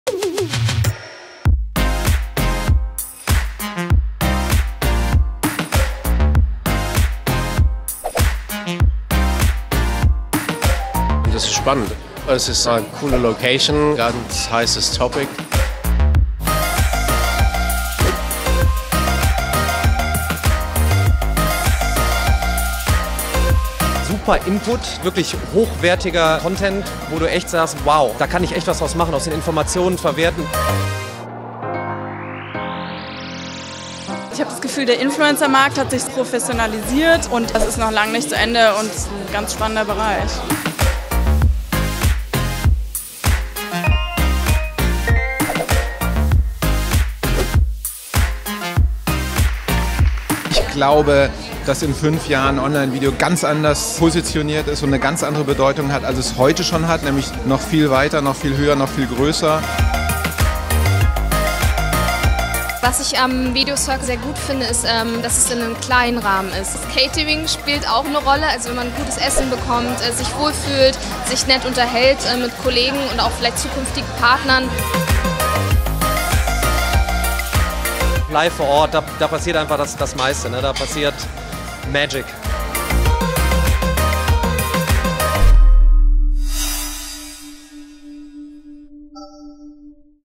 Video Circle Event #1 Berlin I Aftermovie feat. Twitch, Jolina Mennen, Daniel Jung, Tubular
Am 15. Juni 2018 war es so weit: Der erste Video Circle feierte  Premiere in Berlin. In einer exklusiven Location und ausgewähltem Gästekreis kamen über 50 Branchenexperten zusammen, um sich zu vernetzen und über die jüngsten Trends und Entwicklungen im Online-Video-Markt auszutauschen.

Aufregenden Input und zahlreiche Insights lieferten aufregende Gastspeaker wie

Christophe Gobel, Sales Director EMEA Tubular Labs
Burkhard Leimbrock, Commercial Director Twitch
Manuel Challal, Head of Client Services Brandboost by Divimove
Daniel Jung, YouTube-Creator und Mathe-Rockstar
Jolina Mennen, YouTube-Creator und Beauty-Ikone